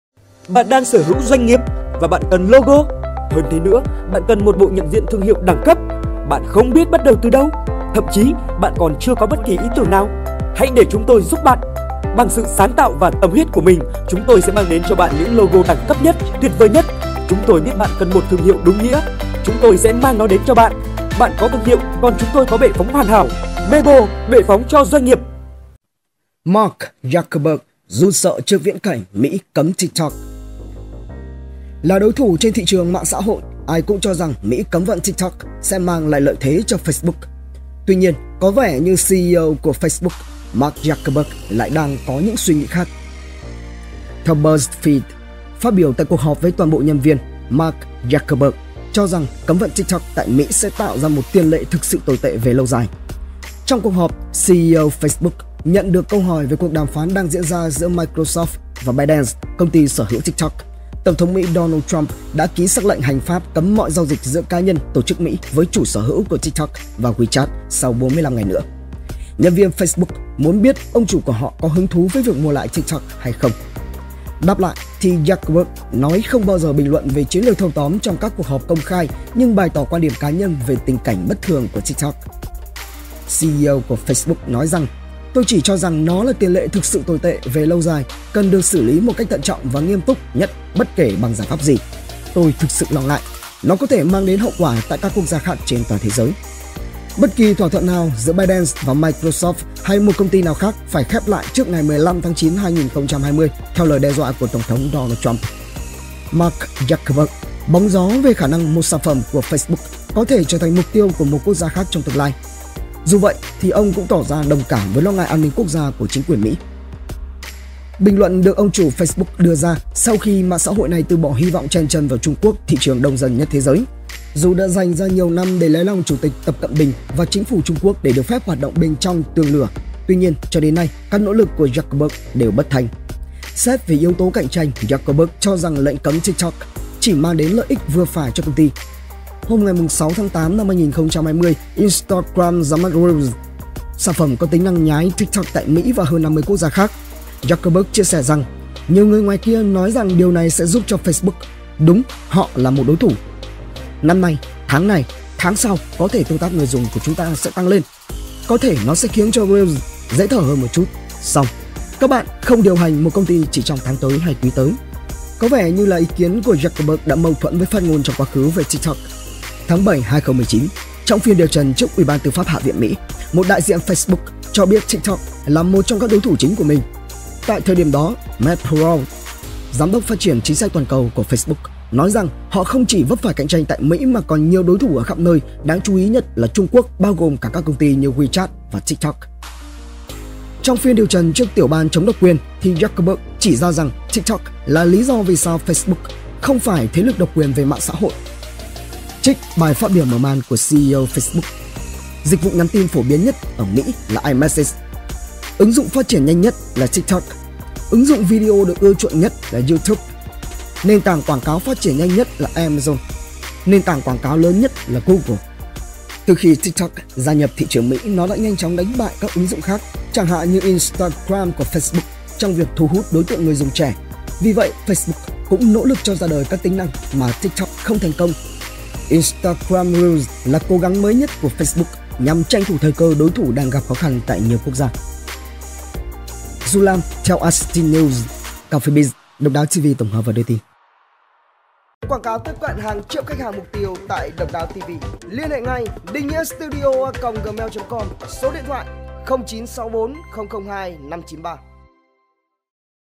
TikTok Bị Cấm, CEO Facebook Vui Hay Buồn? | TIN NÓNG CÔNG NGHỆ
► NHẬN TƯ VẤN THIẾT KẾ LOGO VÀ NHẬN DIỆN THƯƠNG HIỆU TẠI ĐÂY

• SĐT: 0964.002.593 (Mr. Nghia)

Theo BuzzFeed, phát biểu tại cuộc họp với toàn bộ nhân viên, Zuckerberg cho rằng cấm TikTok tại Mỹ sẽ tạo ra “tiền lệ thực sự tồi tệ về lâu dài”. Trong cuộc họp, CEO Facebook nhận được câu hỏi về cuộc đàm phán đang diễn ra giữa Microsoft và ByteDance – công ty sở hữu TikTok. Tổng thống Donald Trump hôm nay ký sắc lệnh hành pháp cấm mọi giao dịch giữa cá nhân, tổ chức Mỹ với chủ sở hữu của TikTok và WeChat sau 45 ngày nữa. Nhân viên Facebook muốn biết ông chủ của họ có hứng thú với việc mua lại TikTok không